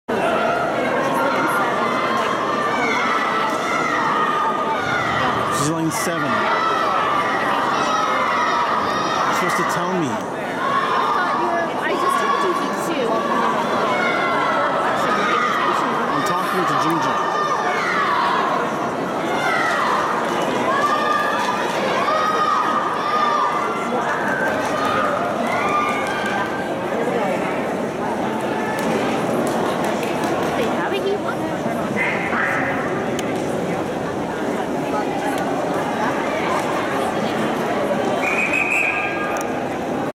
2018/07/14 Briseis 100M Breast 12&U Girls 2018 UCSC Western Finale E15H2
2018 Jul 14 8:45am

Briseis Wu
100M Breaststroke
Prelim
OSC
Event 15
Heat 2
Lane 7

Seed Time - 1:36.16s
Final Time - 1:41.99s
Added +5.83s

Heat Placed - 7th
Final Placed - 11th

splits
50M - 48.03s
100M - 53.96s

Madison Machan - Lane 1
Michelle Michael-Ailuelohia - Lane 2
 - Lane 3
Payton Sheehan - Lane 4
Ceilidh McKegney - Lane 5
Sarah Caparino - Lane 6
Briseis Wu - Lane 7
Meredith Chicilo - Lane 8